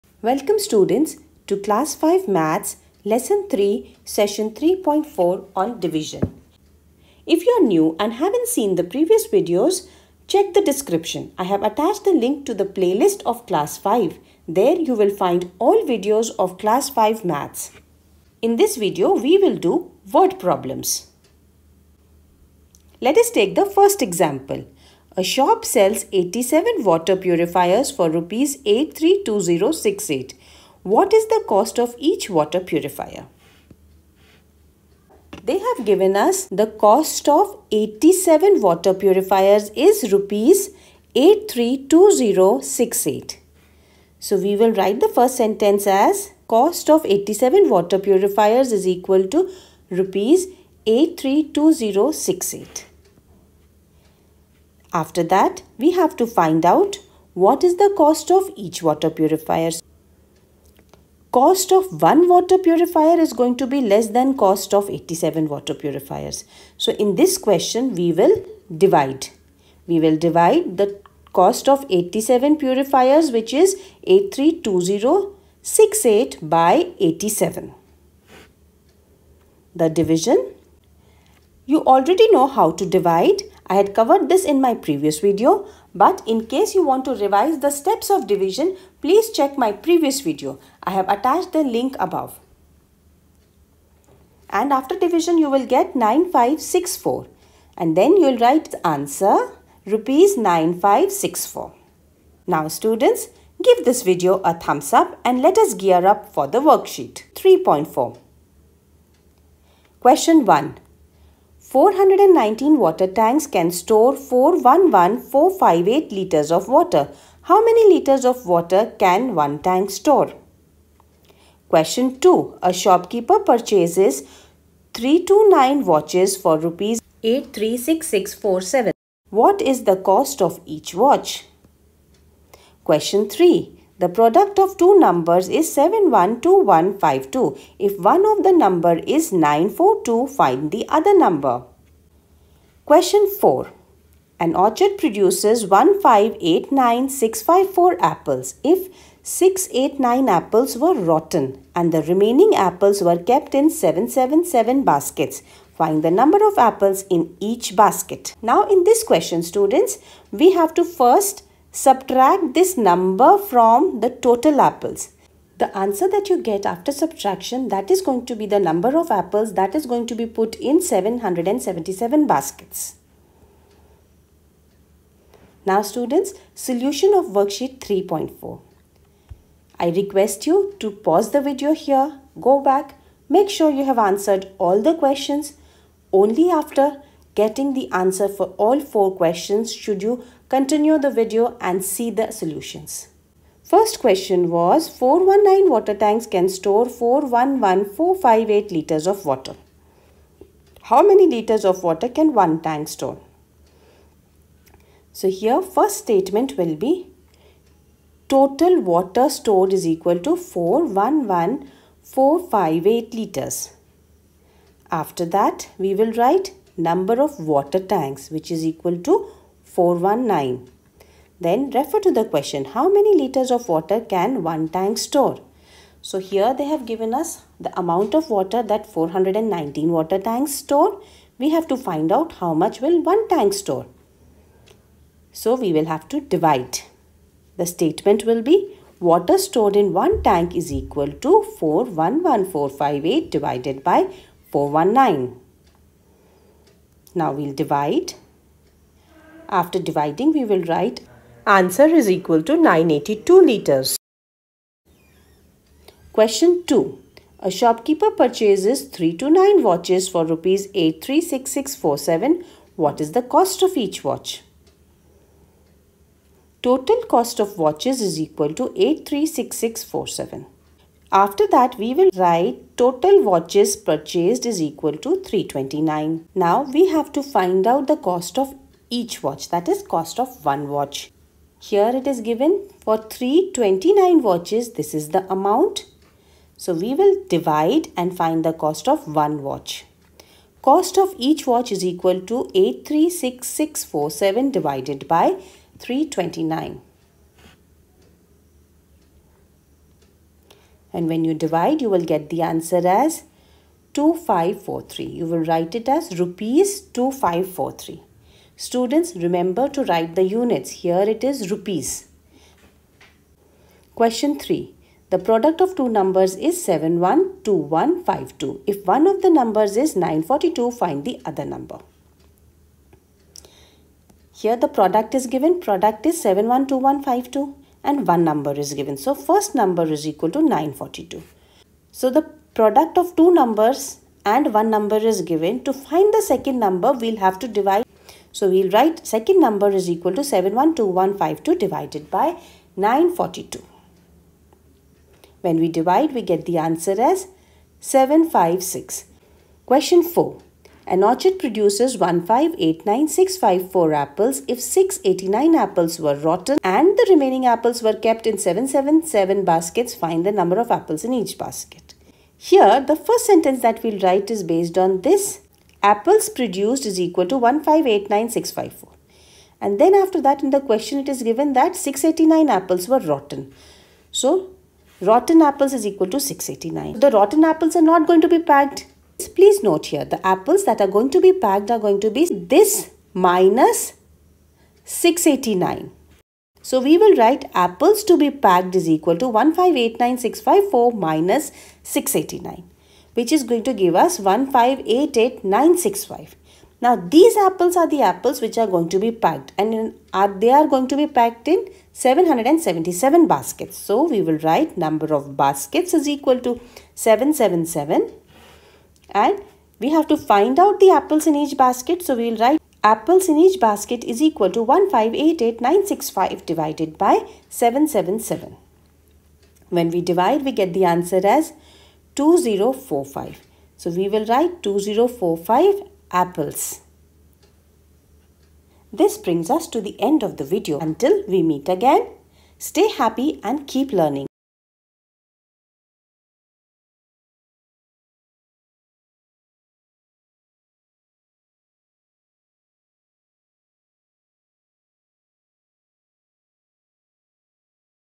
Division Story Problems | Grade 5 | Ch 3, Session 3.4
In this video you will find division word problems for 5th grade. Word Problems are also called story problems.

Do check out the following link, which covers the entire syllabus of class 5 Maths with worksheets and solutions to the worksheets.